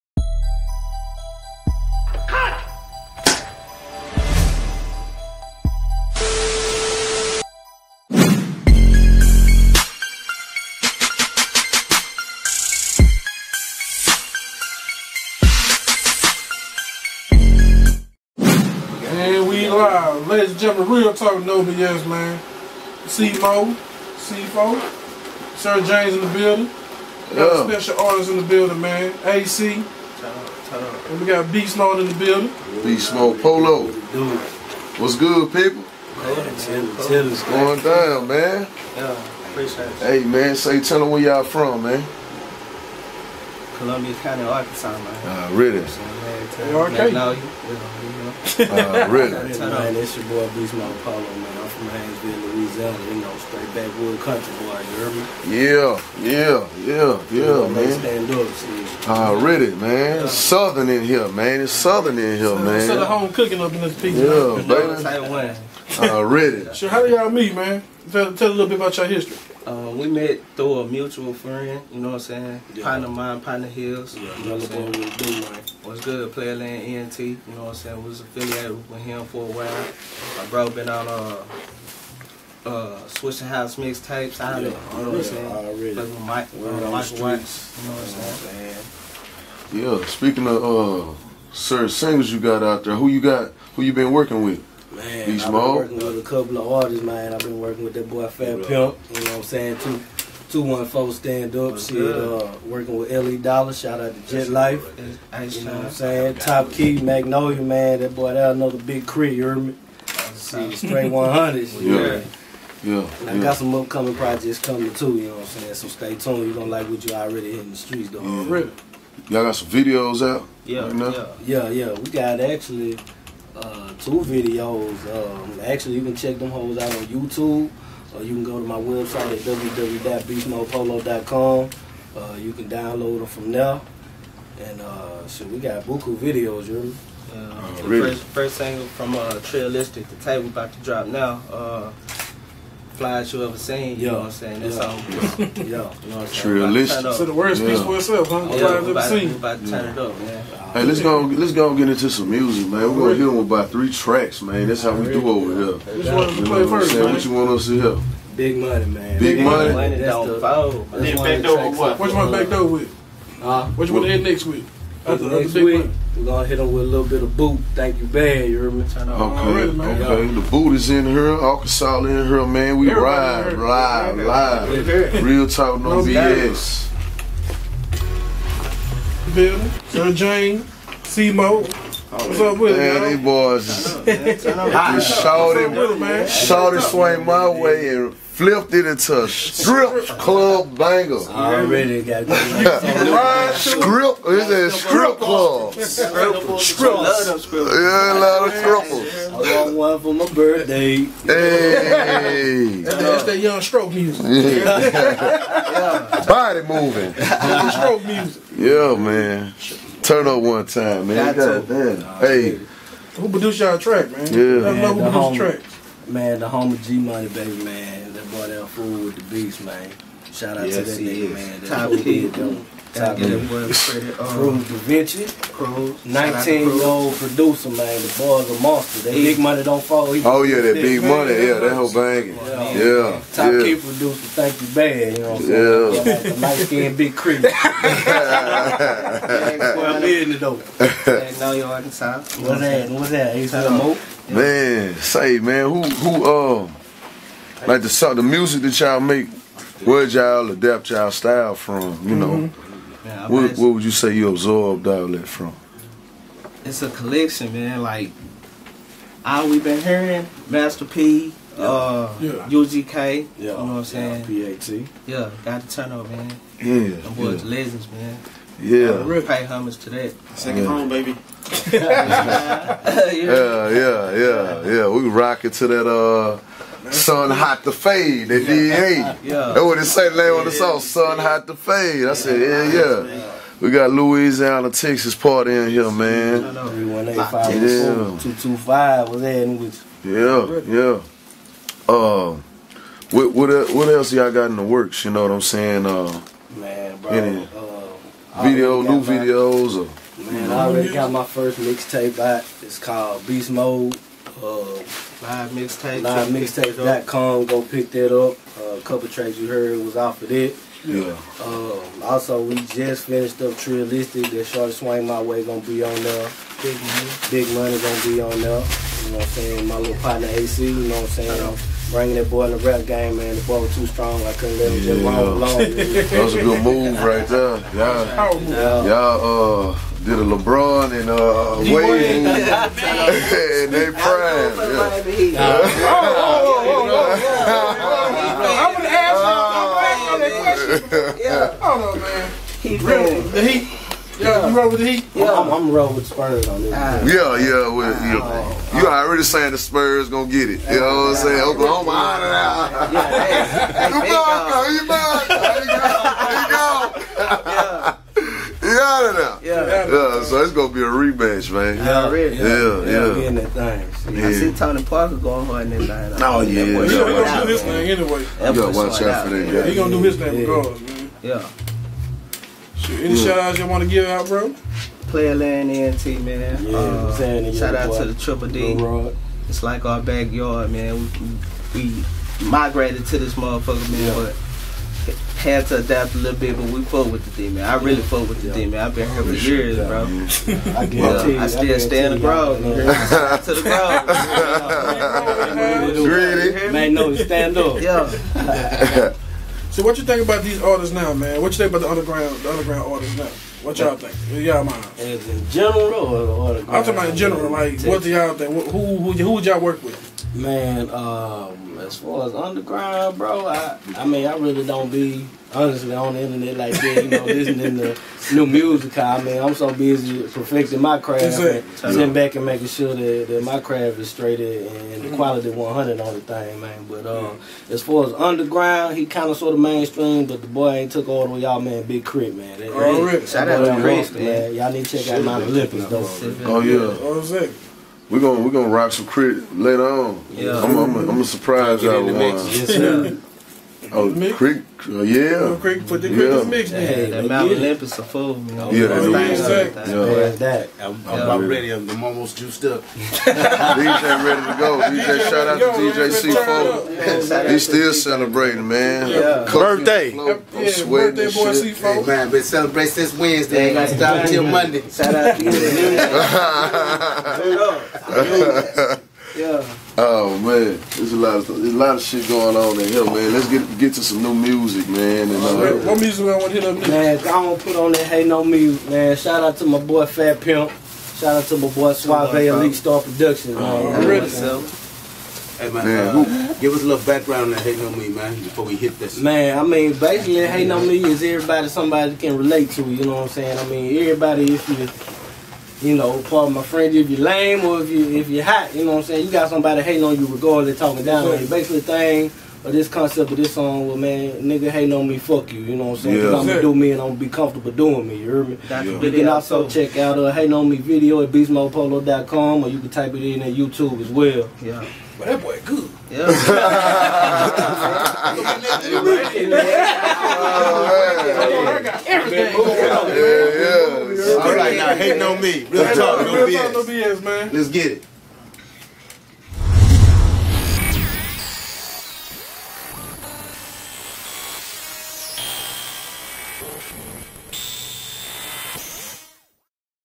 Realtalk NO BS - Beastmode Polo & Aye Cee (05/23/2014 Show)
Filmed By: Kentrell Stepney (@Trill_A_G)
Edited By: Jeffrey Oyeneye (@jeffgotbeats)

Realtalk NO BS interviews Beastmode Polo & Aye Cee about what they have going on.